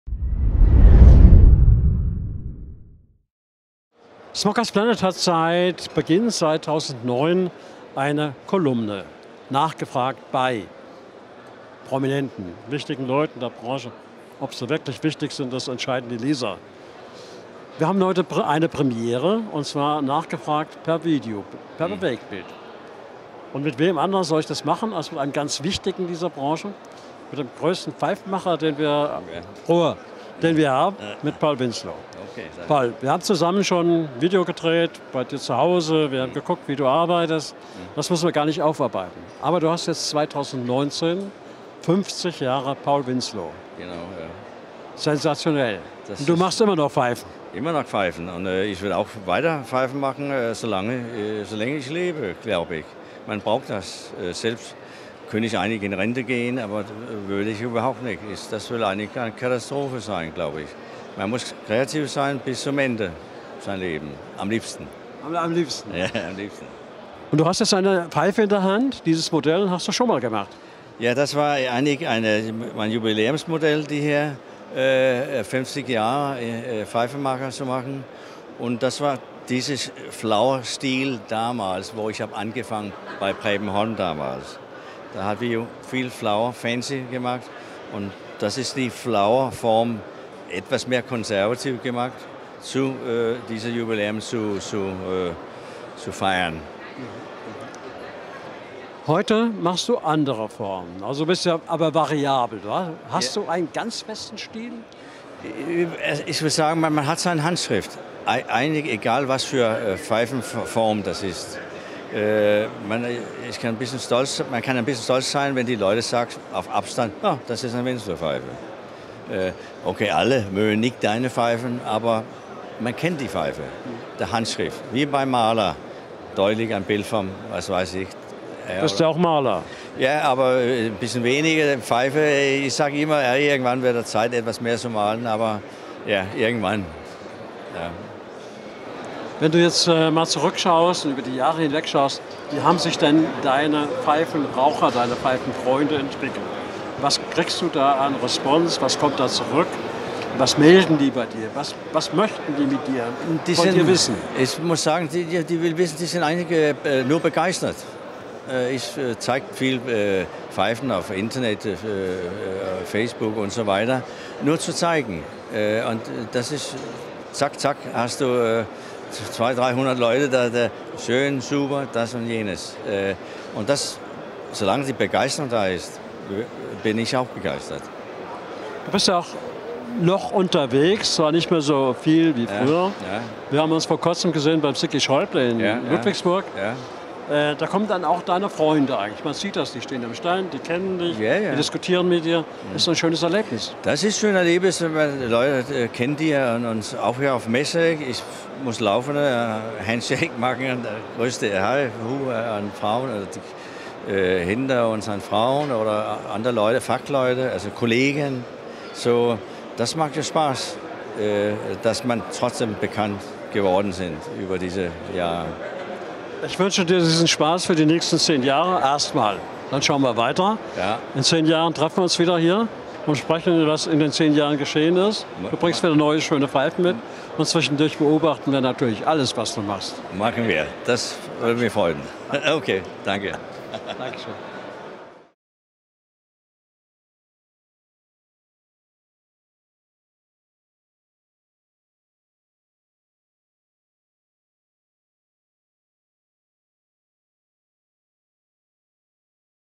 Nachgefragt bei Poul Winsløw
Poul Winsløw, der Künstler unter den genialen Pfeifenmachern, ist der Premieren-Mann der neuen Nachgefragt-Generation auf smokersplanet.de
50 Jahre Poul Winsløw: Ein außergewöhnlicher Pfeifenmacher, ein außergewöhnlicher Mensch, eine außergewöhnliche Geschichte - hier auf smokersplanet.de